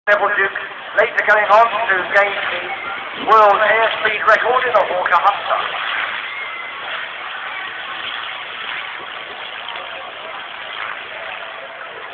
Sunderland Air Show 2010
Video Taken While I Was At Sunderland Air Show

Yes It Was Taken Off A Mobile Phone & Its Bad Quality

I Will Find The Names of The Planes & Helicopters When I Have Time!